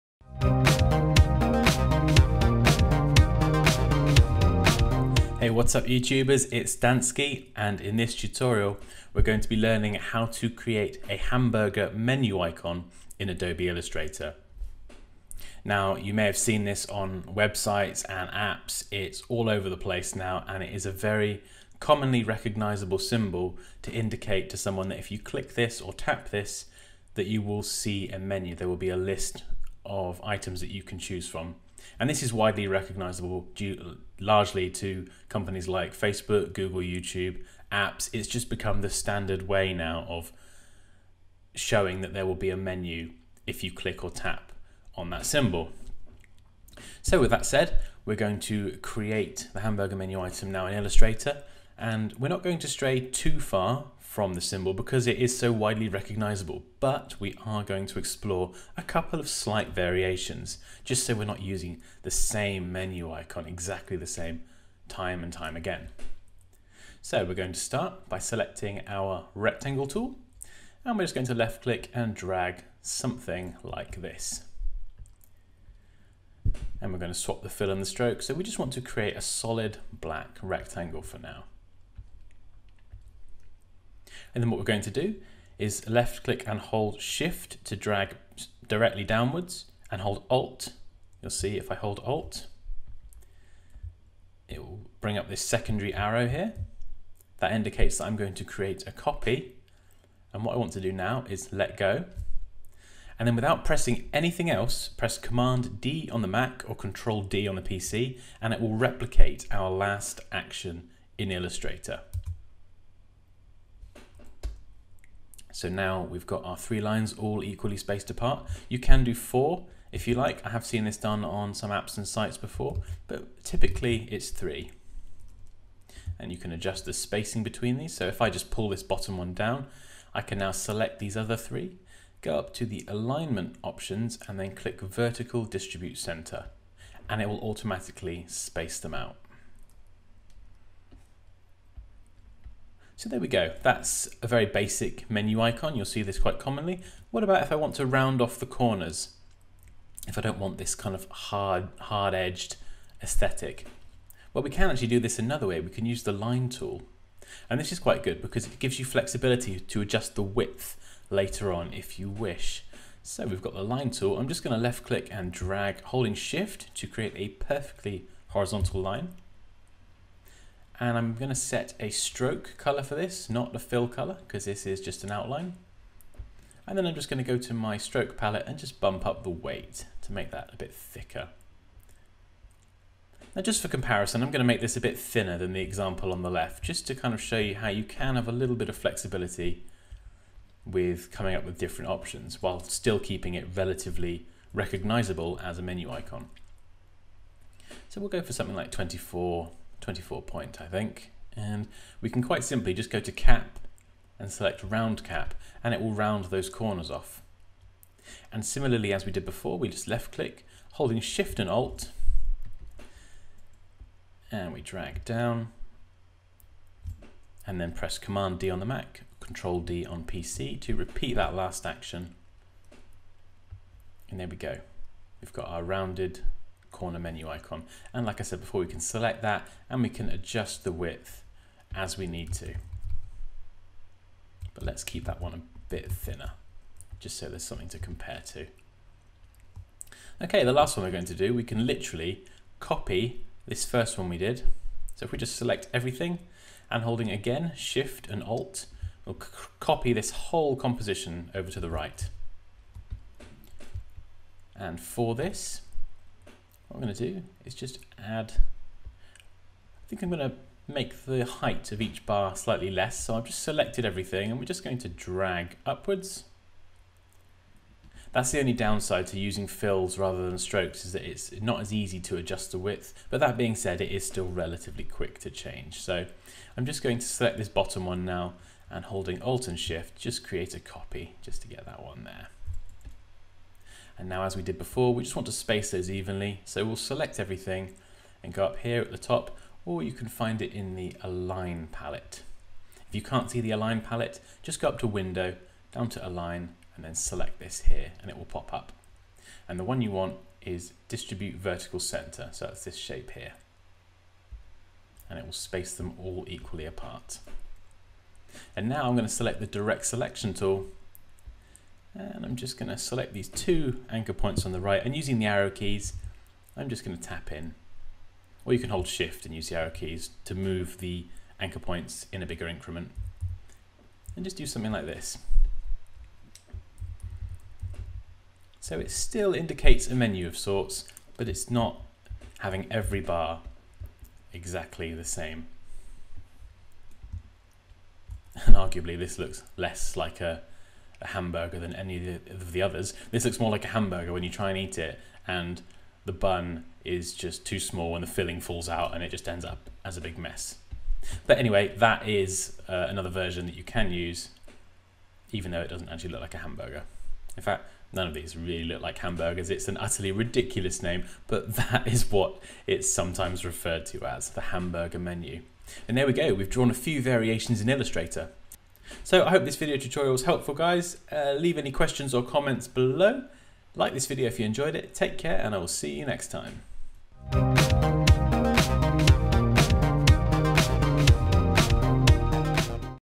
Learn How to Draw 3 Hamburger Menu Icons in Adobe Illustrator | Dansky
In this tutorial, we're going to learn how to draw 3 different hamburger menu icons in Adobe Illustrator.

➡️ Want to level-up your designs with millions of creative assets?

👥 Join me and other designers in our free private community